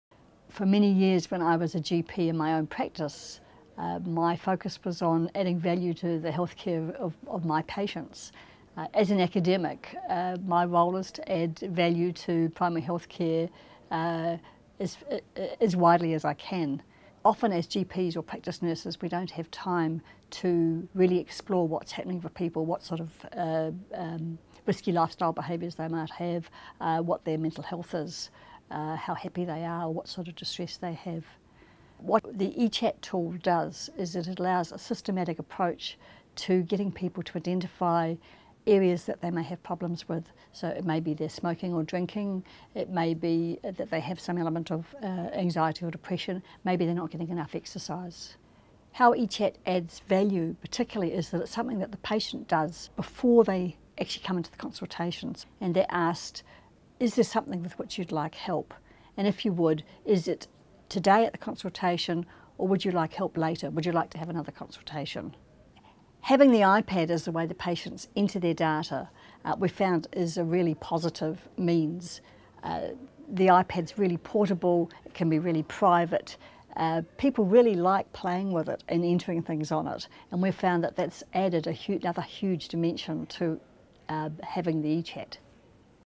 Felicity Goodyear-Smith talks eCHAT
In a busy GP practice, how do you screen for health and happiness? How do you know who wants help, but doesn't know how to ask?

Prof Felicity Goodyear-Smith is the lead academic on eCHAT, a ground-breaking, license-free screening tool.

eCHAT is available as part of HealthTRx's suite of electronic health screening tools: MyHealthScreenTRx.